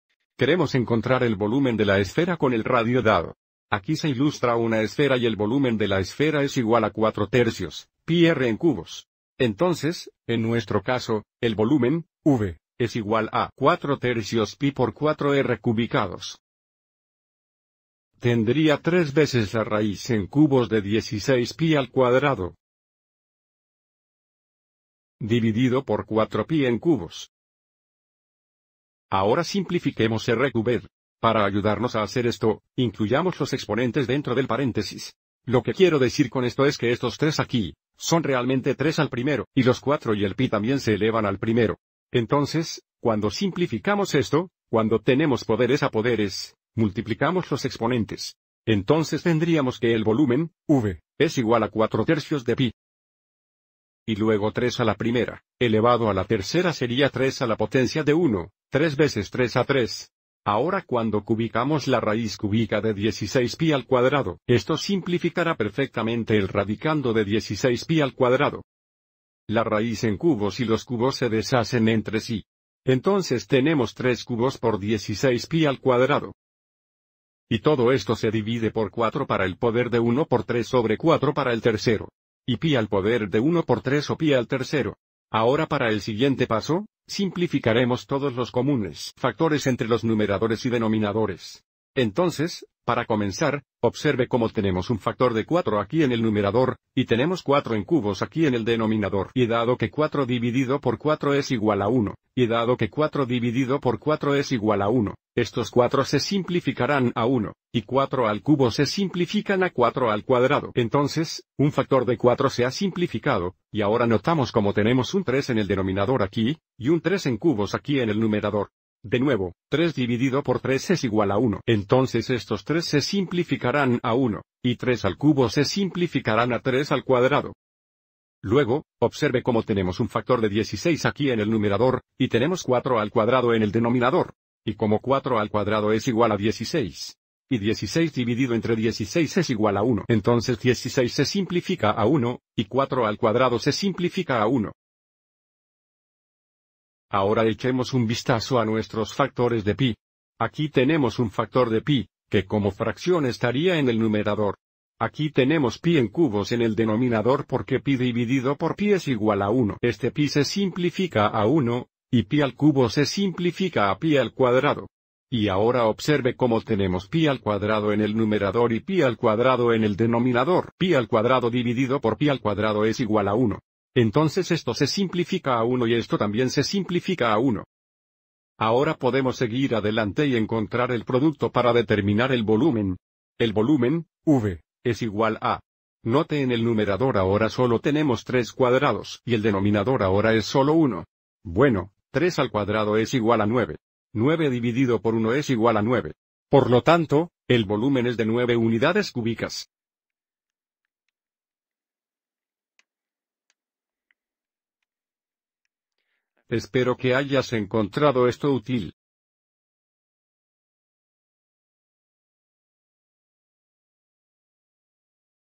Ej: Encuentra el volumen de una esfera cuando el radio es una fracción con un radical
Este video explica cómo encontrar el volumen exacto de una esfera con el radio dado es una Fracción que contiene una raíz cúbica.